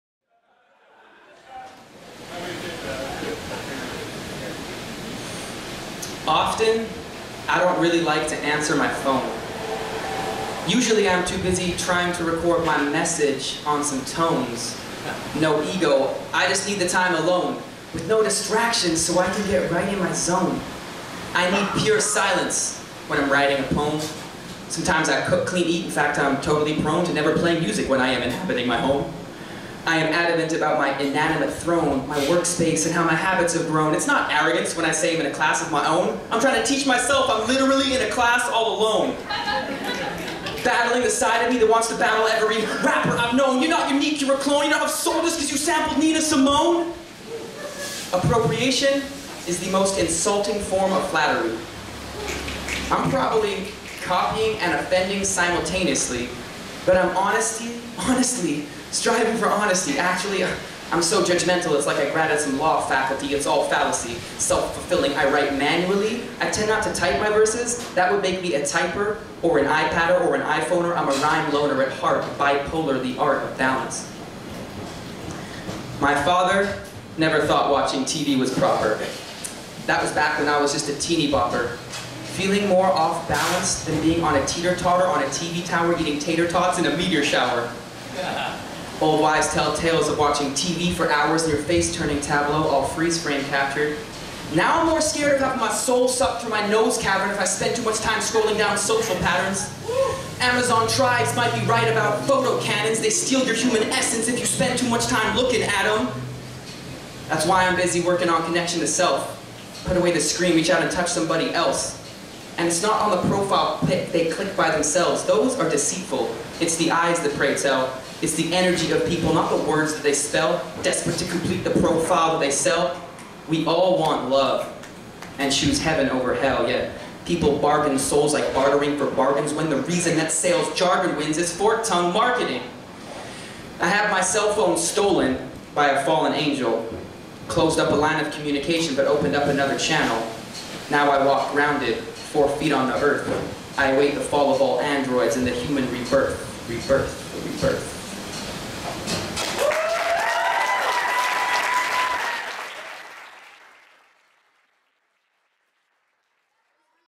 Rupert Common - Phone
Rupert Common competed on April 20th at the first semi-finals for the Vancouver Poetry Slam.  This is Rupert's poem Phone